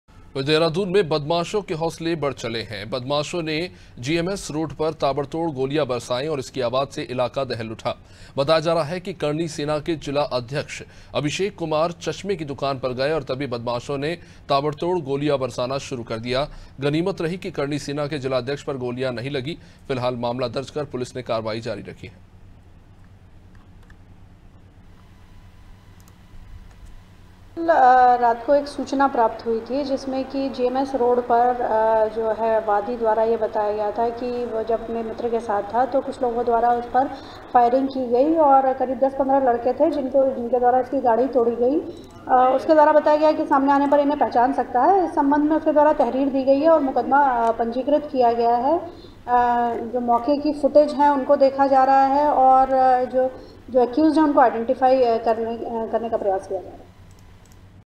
Dehradun : बदमाशों के हौसले बुलंद, बदमाशों ने रोड पर चलाई गोलियां || Anaadi Tv
देहरादून में बदमाशों के हौसले बुलंद होते जा रहे हैं...बदमाशों ने जीएमएस रोड पर ताबड़तोड़ गोलियां चलाई...जिसकी आवाज से इलाका दहक उठा...बताया जा रहा है कि, करणी सेना के जिला अध्यक्ष अभिषेक कुमार चश्मे की दुकान पर गए थे...तभी बदमाशों ने ताबड़तोड़ गोलियां चलाना शुरु कर दी...गनीमत रही कि, करणी सेना के जिलाध्यक्ष पर गोली नहीं लगी...फिलहाल पुलिस ने मामला दर्ज कर लिया है...और सीसीटीवी कैमरों के आधार पर कार्रवाई कर रही हैbreakingnews

COMING SOON
 DEN, AIRTEL